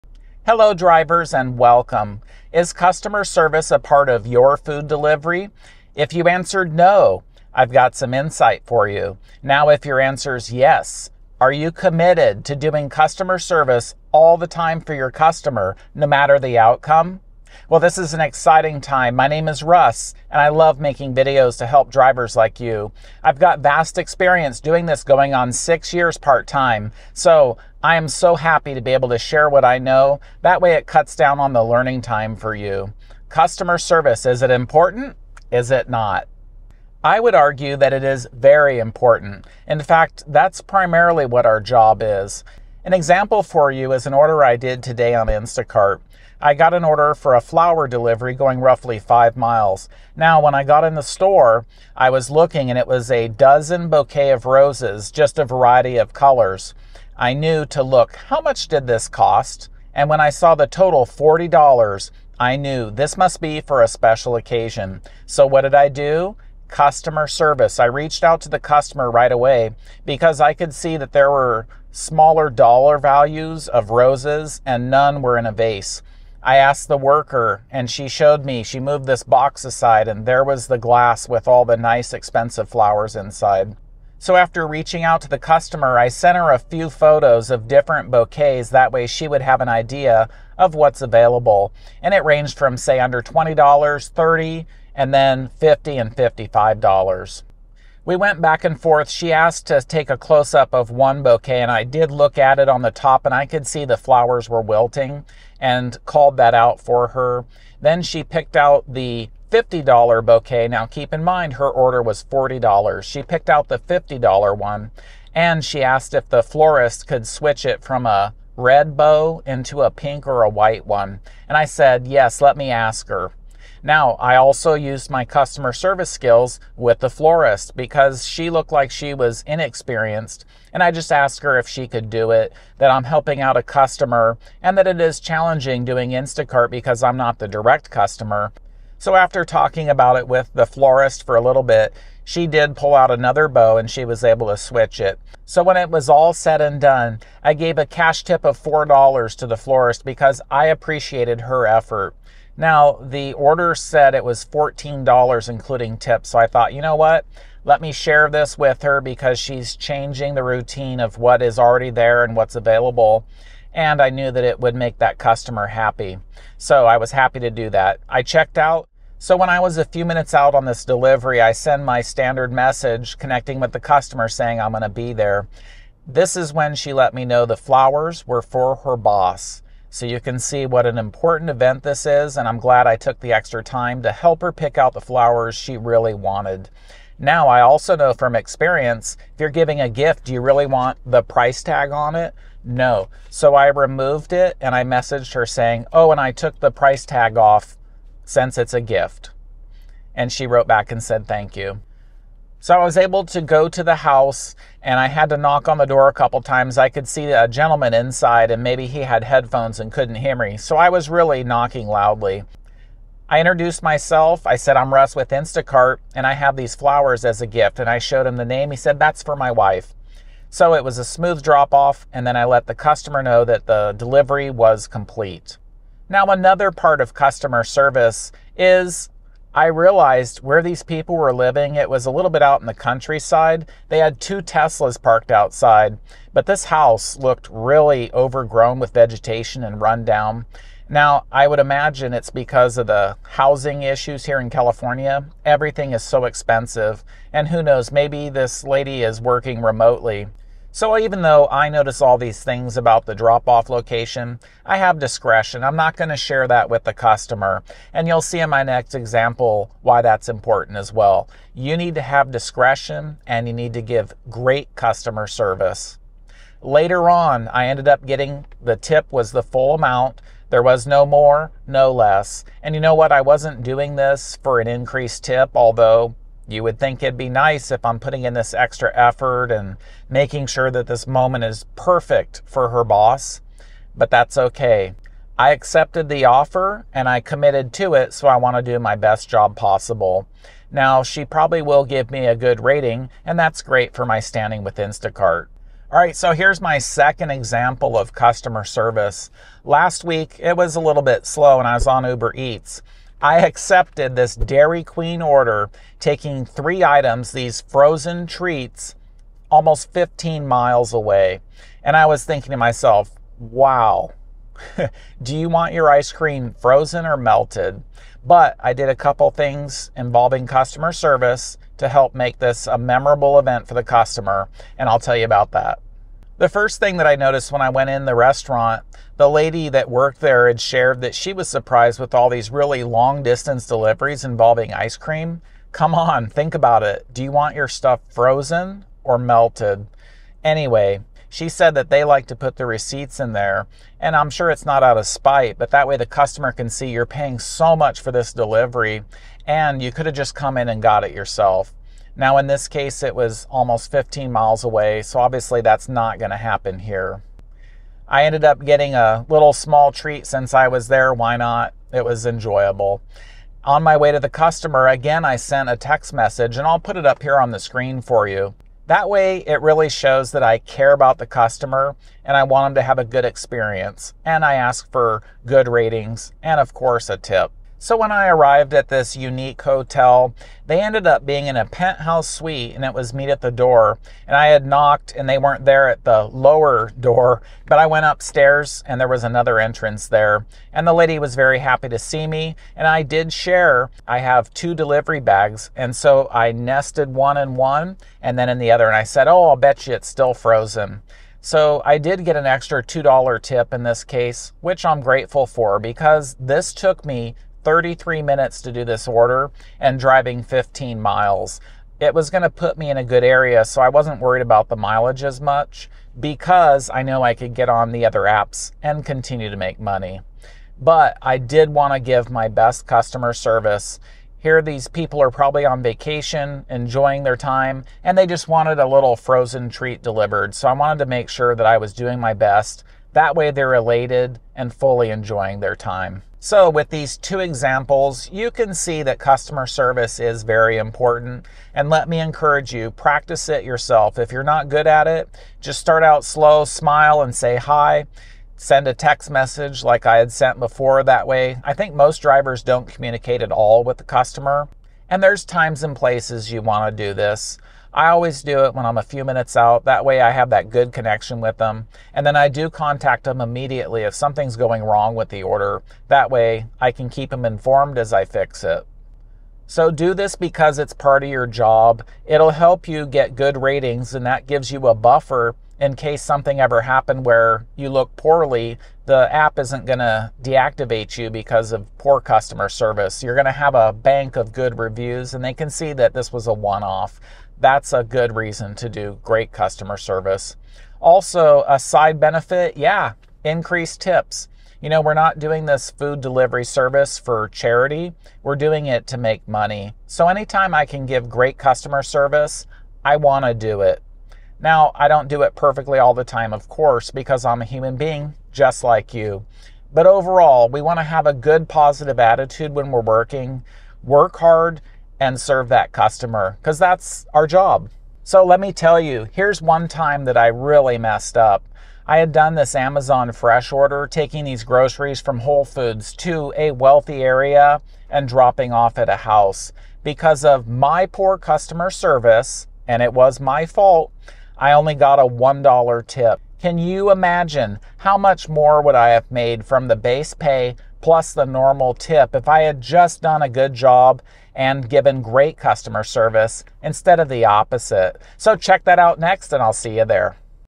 Here's why good customer service is the future (No, Seriously)
Ok, let's find out one way or the other if customer service is really necessary when doing food delivery and even grocery shopping for that matter.  What do you think and why?  What about doing rideshare for Uber and Lyft?  Are there better ways than talking with the customer?  In this video I will address grocery shopping and food delivery.

I know that from time to time I contemplate if I'm wasting my time trying to be nice to customers in order to get a higher tip.  Am I fake?  Or, do I really just try to be as helpful and friendly as possible knowing that I am selecting the best groceries for another human being and their family?  I strive to pick out the best products, even in better condition than I would select for myself.

Do customers wonder how drivers can afford to drive their cars delivering their restaurant food orders?  I'll bet they eat the food in five minutes and it took drivers 20 minutes or more to get it the customer.

Russ